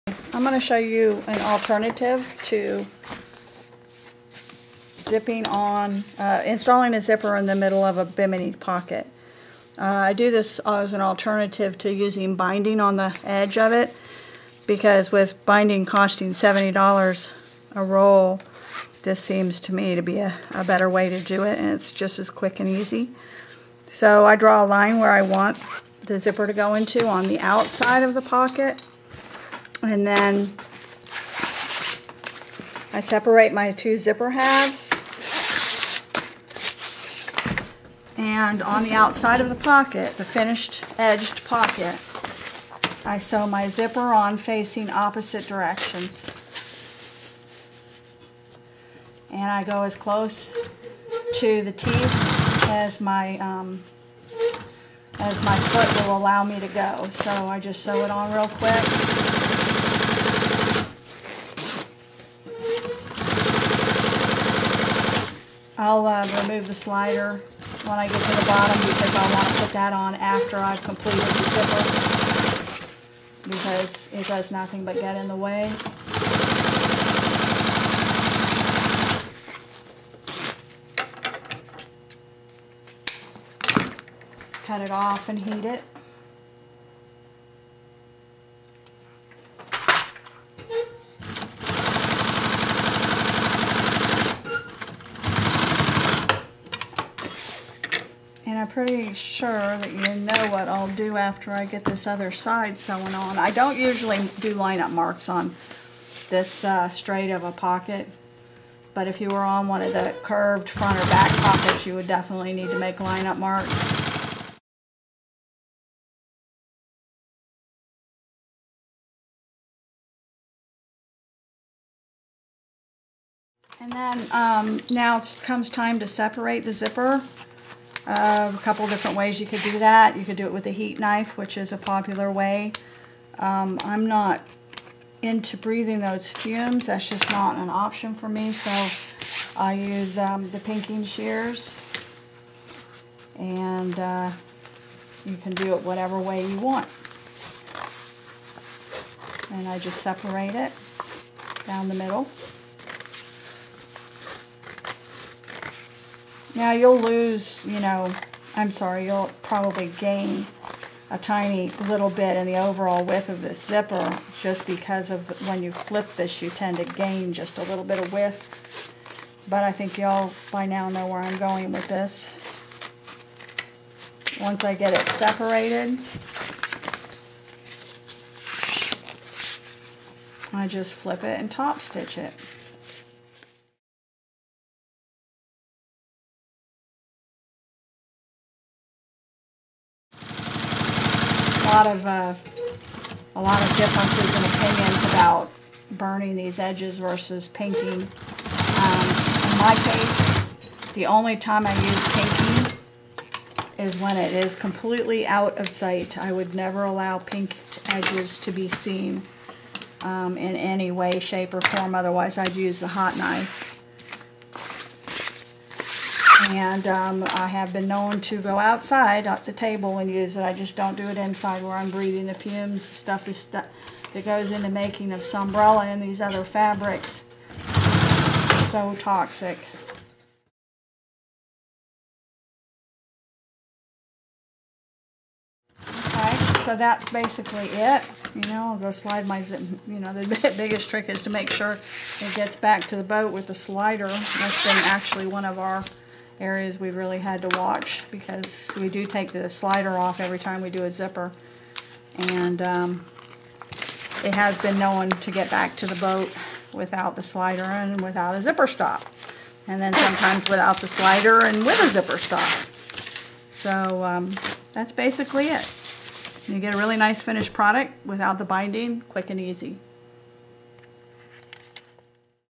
Installing a zipper in a bimini top pocket
An easy way to install a zipper in bimini top pocket